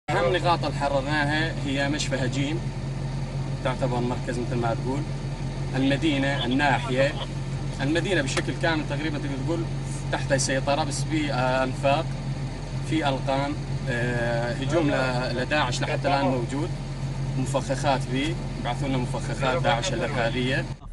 بعد أن ارتكب داعش بحقهم المجازر.. أبناء الشعيطات يشاركون في تحرير هجين
شاهد آيضا:
قيادي بعثي يفضح تاريخ آل الأسد!؟
بعد أن ارتكب داعش بحقهم المجازر.. أبناء الشعيطات يشاركون في تحرير

في رحلة محفوفة بالمخاطر اتجهنا الى بلدة هجين كان علينا ان نستقل احد العربات المصفحة لنتوغل داخل هجين برفقة احمد الذي يشرح لنا بعض مجريات المعركة.

يقول أحمد الشيعطات المقاتل من قوات سوريا الديمقراطية: "اهم النقاط التي قمنا بتحريرها هي المشفى والسوق داخل البلدة حيث تعتبر المركز ويستخدم التنظيم الالغام والمفخخات التي تعيق تقدمنا الا ان البلدة تحت السيطرة النسبية".

مشاهد الدمار اصبحت مألوفة بعد معارك عنيفة بين قوات سوريا الديمقراطية ووتنظيم داعش. بالقرب من احد البيوت يقف رفعت مع رفاقه بالقرب من نار اوقدها ليحصل على كوب من الشاي في استراحة مقاتل.

من جانبه يضيف رفعت علي المقاتل من قوات سوريا الديمقراطية: "نحن الان متواجدين وسط بلدة هجين والقوات تتوجه الى قرية الموحسن وفي الصباح قامت مجموعة مؤلفة من عشرين مقاتل من داعش بمهاجمتنا الا اننا قمنا بالتصدي لهم".

وفي جهة اخرى يساعد روجهات رفافه لاعداد الذخيرة استعدادا للتوجه الى الخطوط الامامية للمعركة.

ويقول روجهات تلقيادي في مجلس ديرالزور العسكري: "دخلنا من محور ابوحمام وواجهنا هجوم قوي عنيف من قبل داعش بالمفخخات والان نحن نتمركز داخل هجين في السوق والحملة مستمرة حتى القضاء على اخر داعشي".

معركة هجين التي لم تحسم بعد بسبب مقاومة التنظيم الشرسة للدفاع عن أخر جيوبه شرق الفرات، حيث استطاعت قوات سوريا الديمقراطية تحرير اهم النقاط ضمن البلدة ومن بينها المشفى والسوق المركزي داخل هجين.